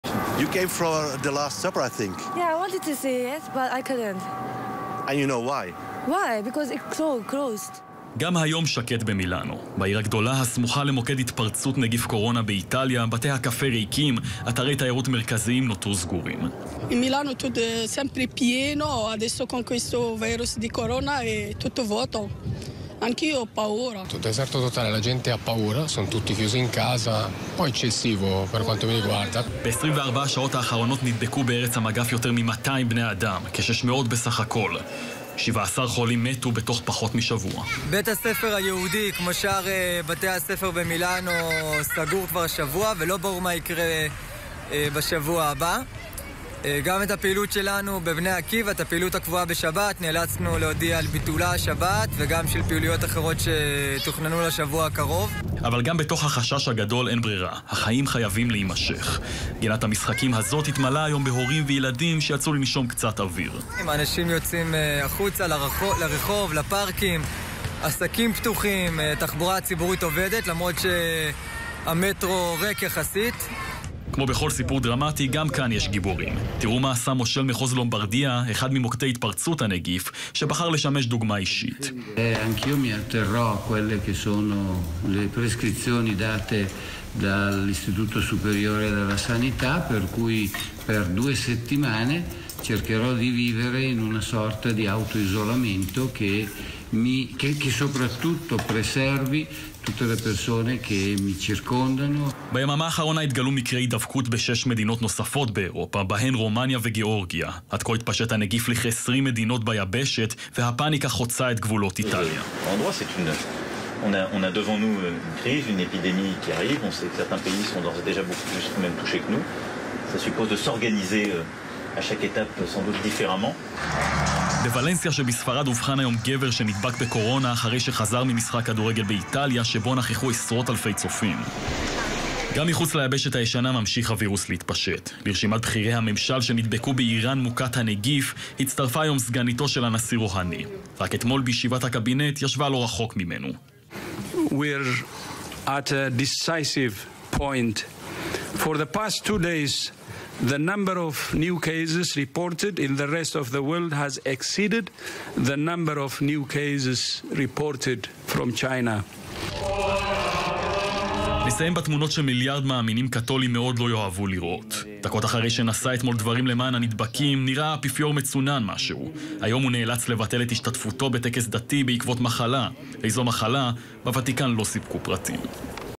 הנגיף הסיני ממשיך לפגוע בבכירים איראנים
כאן חדשות | בד בבד, הווירוס ממשיך להתפשט ברחבי אירופה. איטליה היא מוקד ההתפרצות הגדול כרגע מבין כ-20 מדינות ביבשת. נשיא צרפת מזהיר ממגפה. כתבתו של יואב זהבי, מתוך חדשות הערב 27.02.20